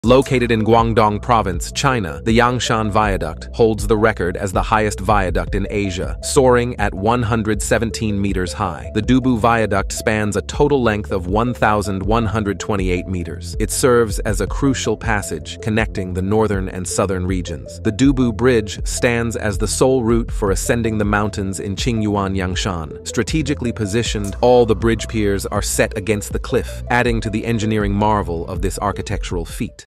The Highest Bridge in Asia.
Located in Guangdong Province, China, the Yangshan Viaduct holds the record as the highest viaduct in Asia, soaring at 117 meters high.
Comprising three bridges, the Dubu Viaduct spans a total length of 1,128 meters. It serves as a crucial passage connecting the northern and southern regions. The Dubu Bridge stands as the sole route for ascending the mountains in Qingyuan Yangshan.
Strategically positioned, all the bridge piers are set against the cliff, adding to the engineering marvel of this architectural feat.

ADDITIONAL READINGS:
Located near Dubu Town, Yangshan County, Qingyuan City, Guangdong Province, Yangshan Viaduct is known as the highest viaduct in Asia. The Dubu Viaduct in Yangshan County will set the highest record in Asia at more than 110 meters, after the original ancient bridge set the record of 70 meters. The new viaduct is divided into two sections: Bridge No. 1 and Bridge No. 2. Bridge No. 1 is about 800 meters long and has eight piers, the distance between piers is 100 meters, the tallest pier is located 126 meters above the ground. The Dubu Viaduct is divided into two groups, each group counts as one lane, both are 2 lanes. The high bridge is towards Hunan and the lower bridge is towards Guangzhou.

The three bridges of the Dubu Viaduct have a total length of 1,128 meters and are divided into three extra-large viaducts, namely the Dubu No. 1, No. 2 and No. 3 extra-large viaducts. The Dubu No. 1 super-large bridge It is built between two mountain canyons, with a total length of 905 meters and a height of 117 meters. It is known as the "highest bridge in southern China" and is the top priority of the entire line. ; the Dubu No. 2 super large bridge surrounds the mountains. It is built with a total length of 1128 meters and a height of 92 meters. Its construction is very difficult. All the docks are on cliffs

There are many quality and safety controls. Points. Construction and coordination are extremely difficult. High technological content and complex construction technology have created the largest highway bridge construction in history in my country.

It spans two mountains and is supported by 52 large columns. It is also the highest viaduct in Asia.